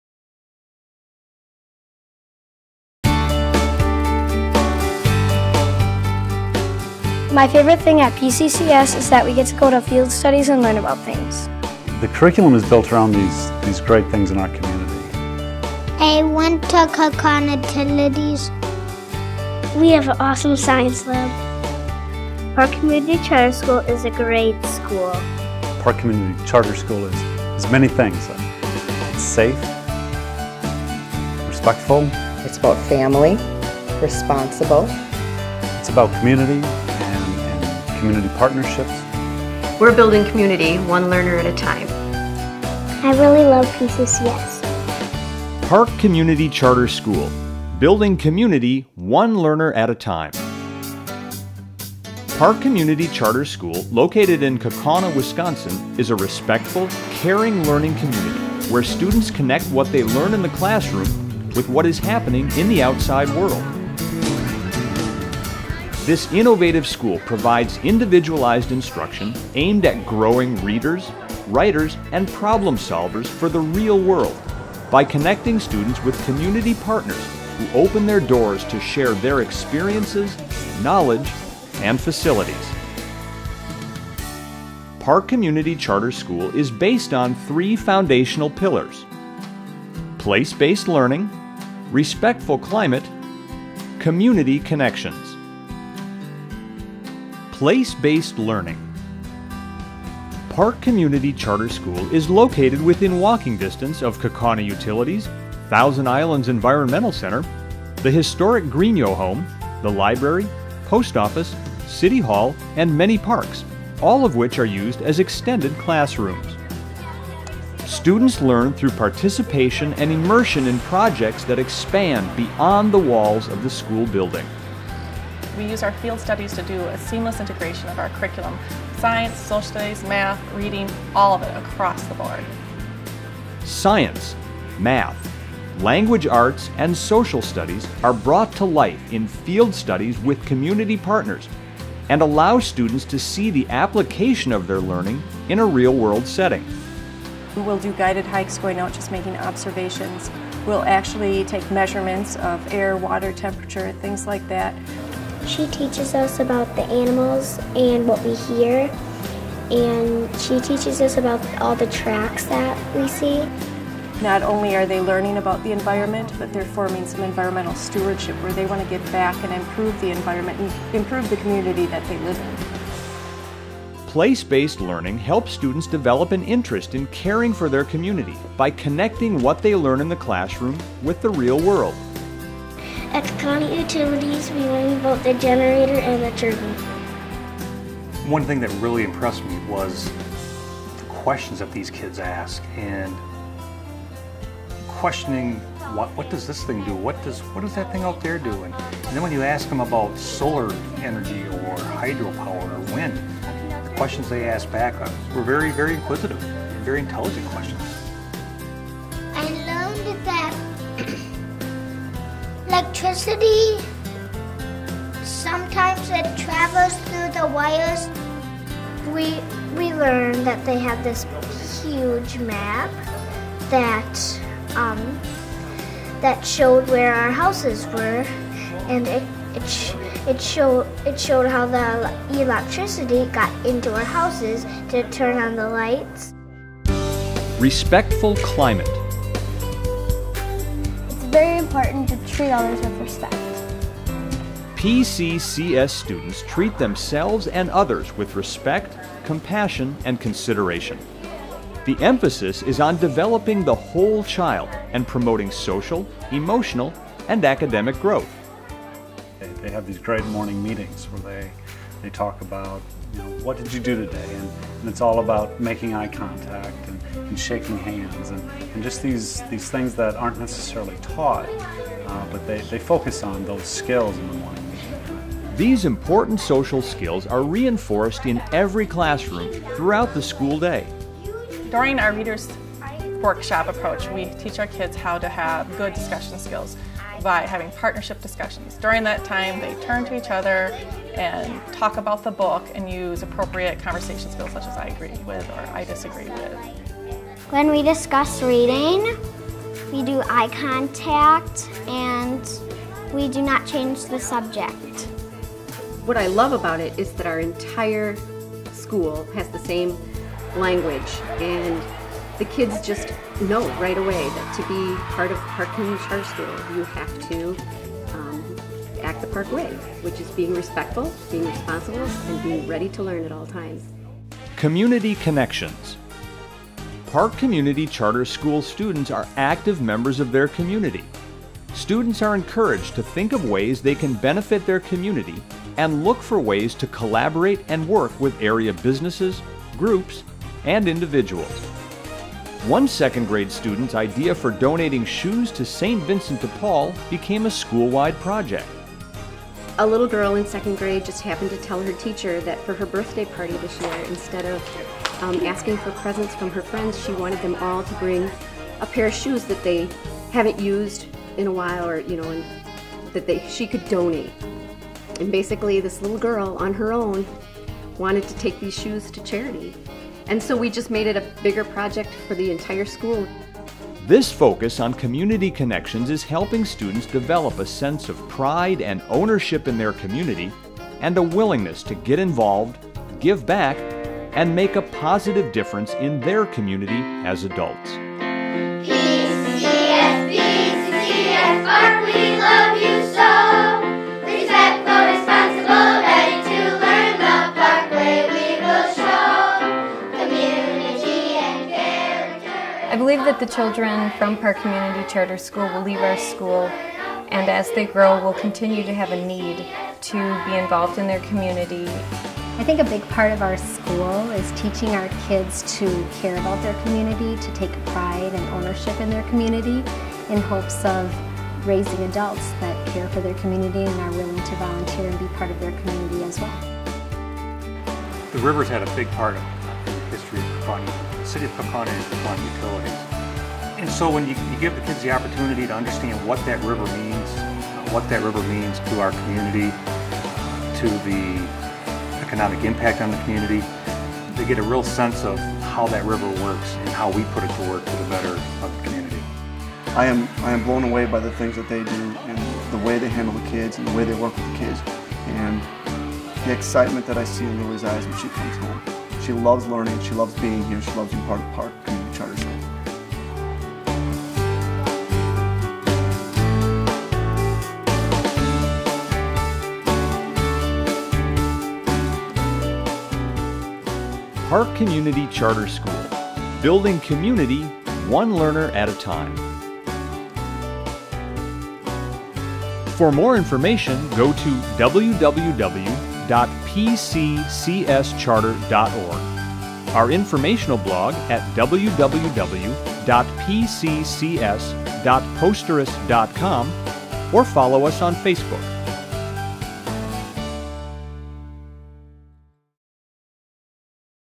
Park Community Charter School
Park Community Charter School (PCCS) is located in Kaukauna, Wisconsin. They strive be involved in the community with hands-on learning at the local nature center and the city's utilities to name just a couple. PCCS created their video to share their passion for their community and their students.